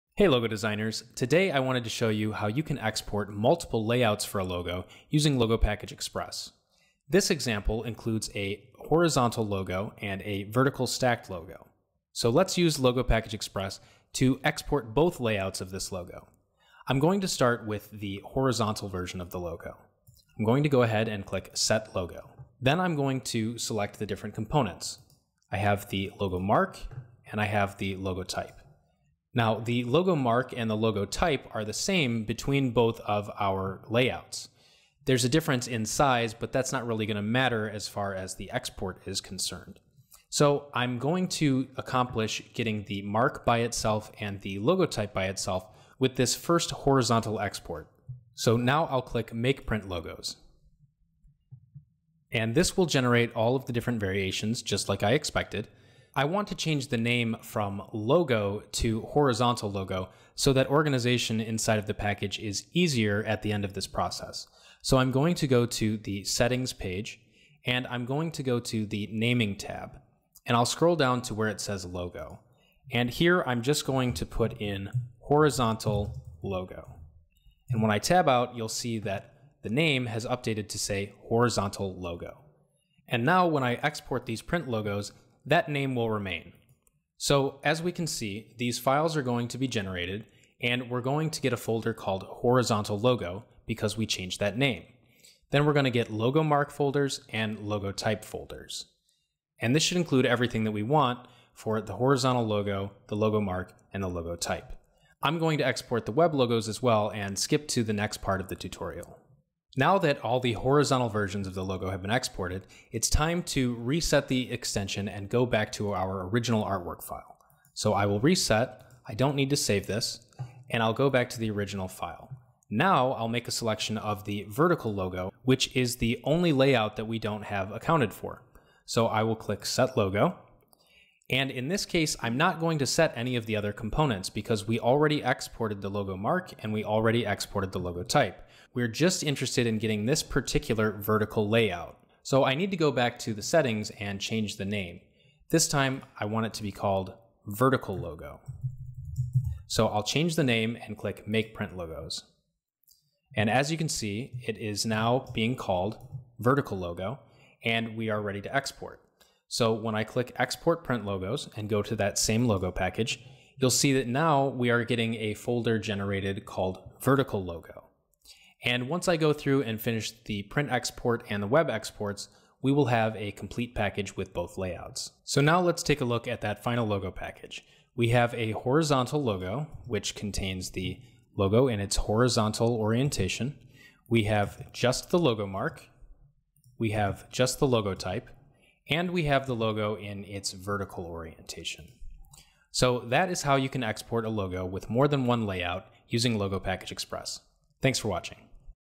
How to Export Different Layouts for Your Logo (Legacy)
(This video references an unsupported version of Logo Package Express. Please visit logopackage.com to upgrade.)

Some brands have more than one layout for their logos. Learn how to make a logo package with multiple layouts using the Logo Package Express Extension for Adobe Illustrator.